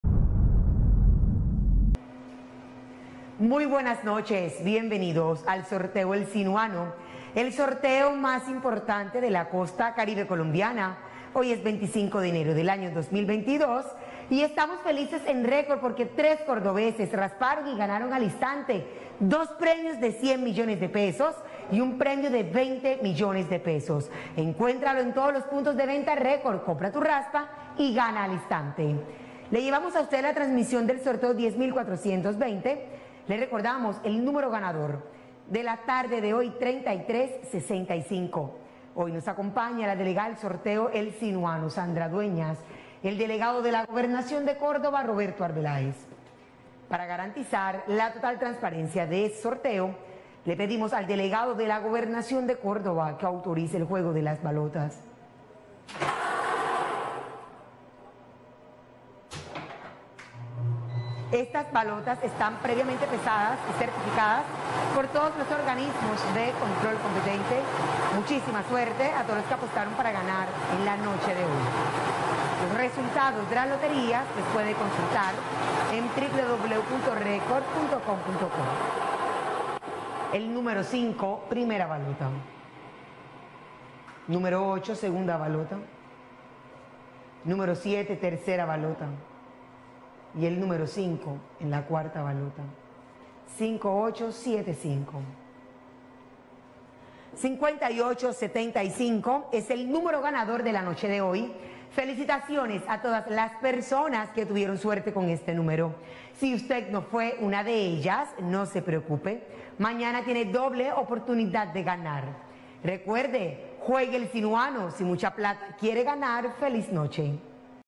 EL SINUANO NOCHE Resultados Martes 25 de enero de 2022 ✅🥇🔥💰
Hola buenas noches, te presentamos los resultados de las loterías y chances en Colombia, vamos con el Resultados del Chance Sinuano noche del Martes 25 de enero de 2022.